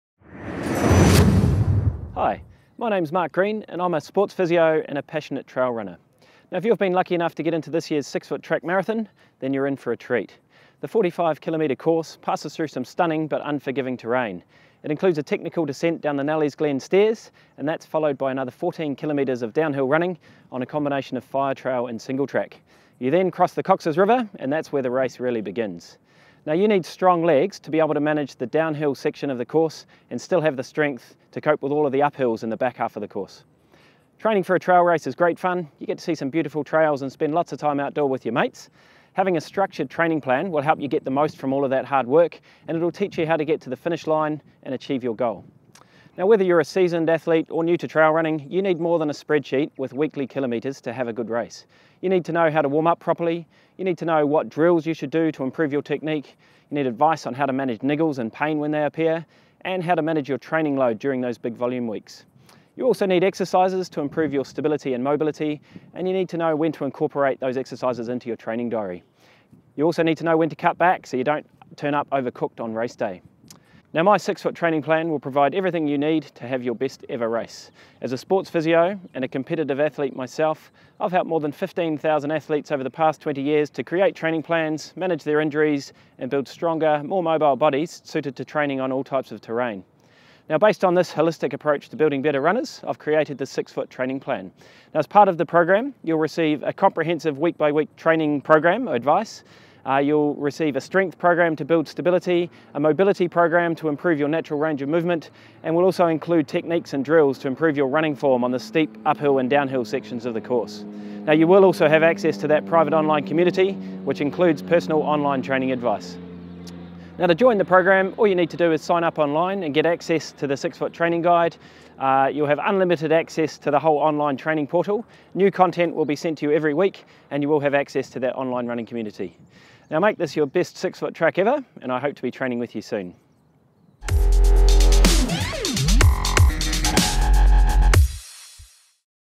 Six Foot Track Marathon Training Plan 2022
Are you racing the 2022 Six Foot Track Marathon? The Locker Room has a training plan specifically designed for this event. Mark Green, a physiotherapist and seasoned trail runner, has finished in the top 4 of the Six Foot Marathon 6 times. He has designed a training program which integrates technique drills, stretching and strengthening exercises, injury management advice and personal online training advice along with his actual mileage training plan to help you have your best Six Foot Marathons race ever!